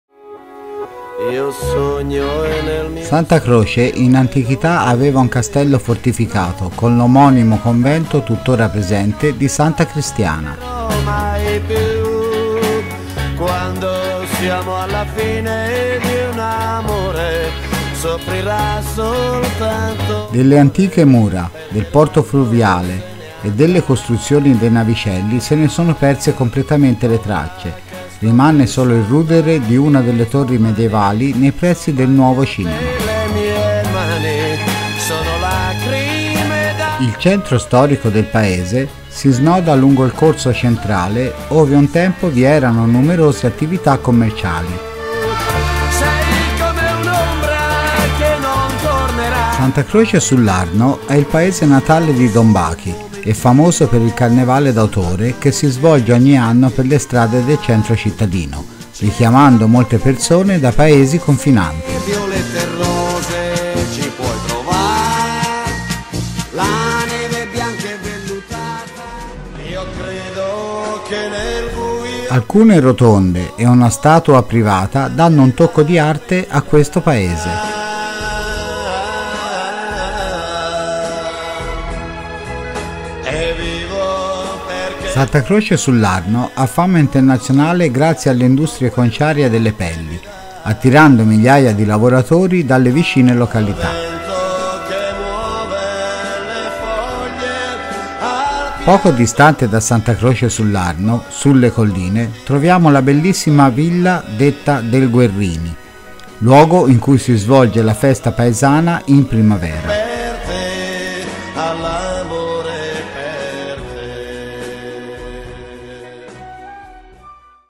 SANTA CROCE SULL'ARNO comune della provincia di Pisa
Santa Croce sull'Arno comune della provincia di Pisa in Toscana. Famoso per la lavorazione delle pelli